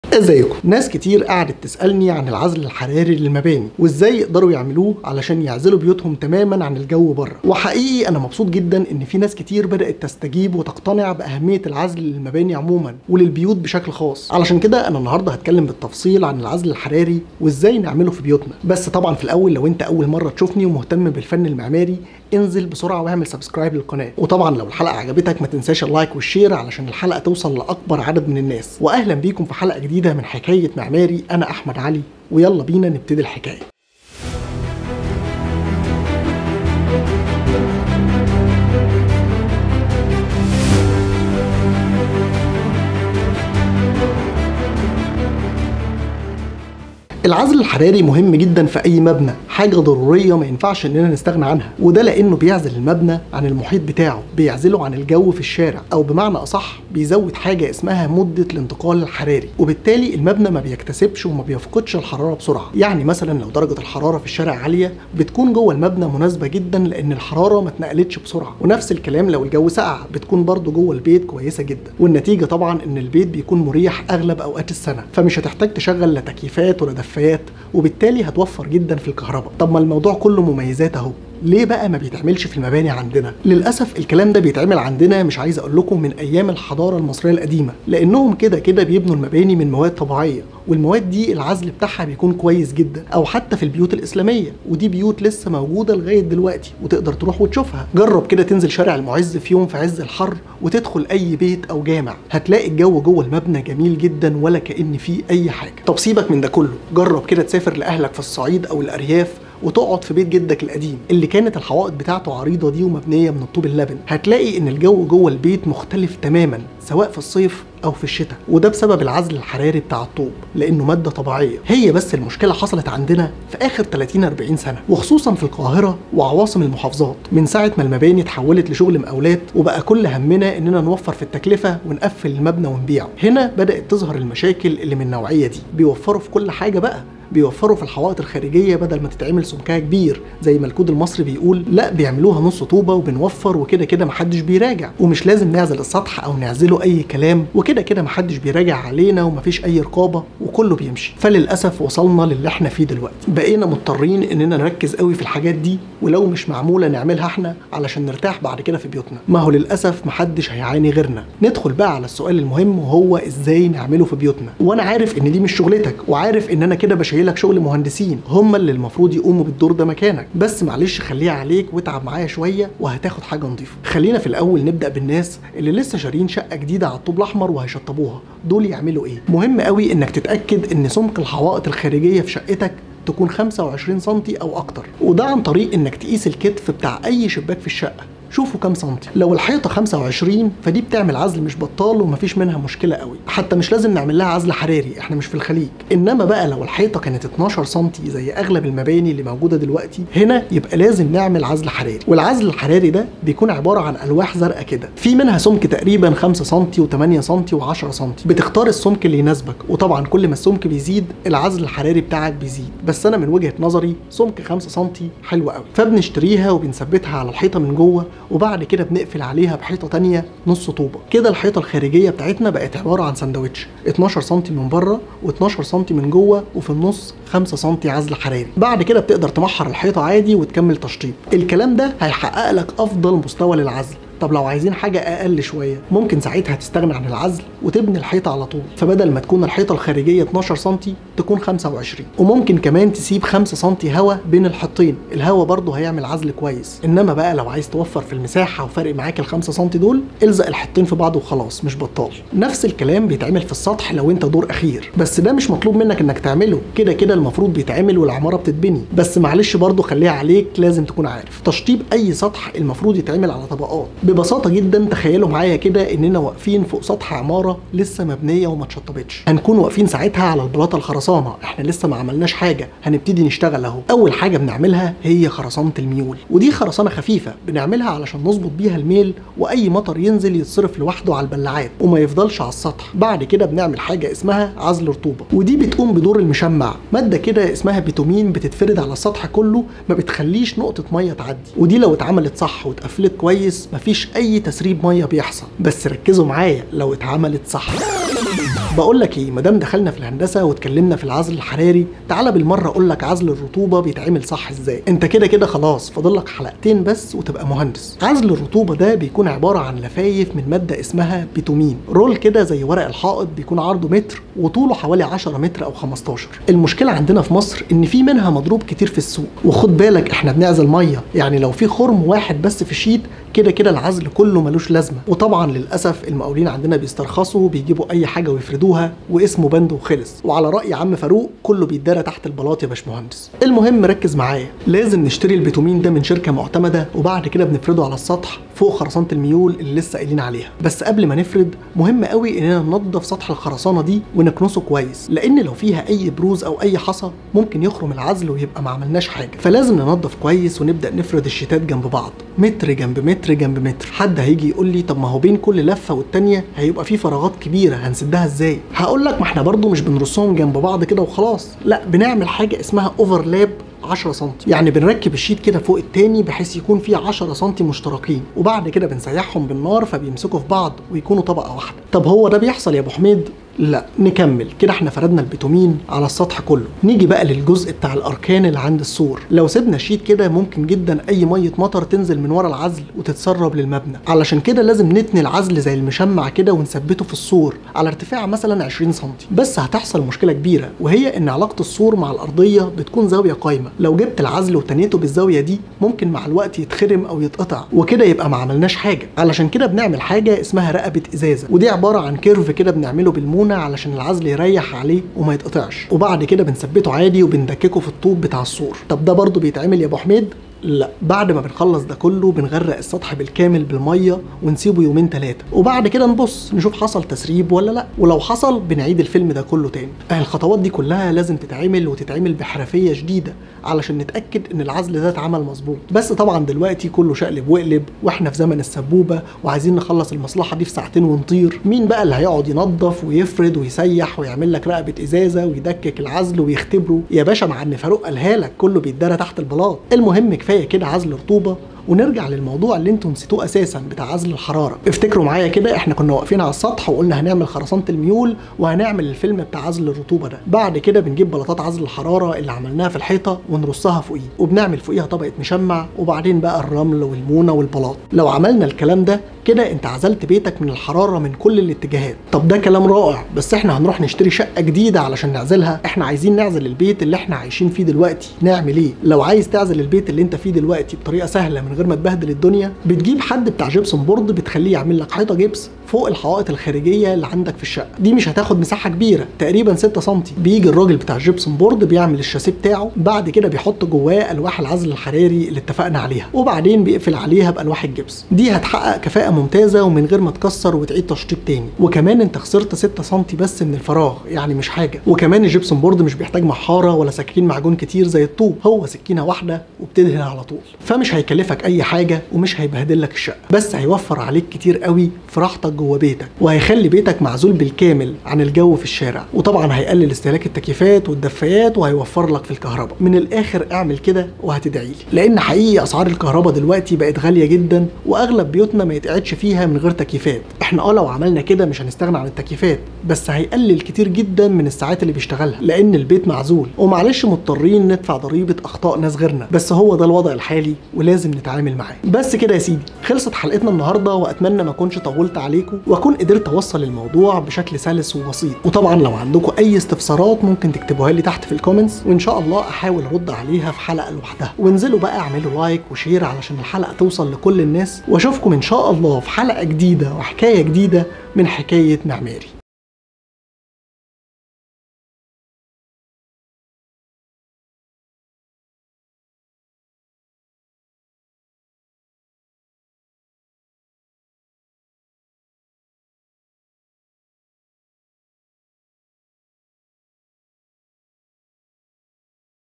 ازاي تخلي بيتك مستدام || العزل الحراري للبيوت
العزل الحراري للبيوت
هنتكلم النهارده عن اول خطوة علشان تخلي بيتك مستدام وهي العزل الحراري. هتعرف ازاي تعمل عزل حراري في بيتك للحوائط والاسقف لو انت دور اخير. بحيث تعزل بيتك تماما عن الجو في الشارع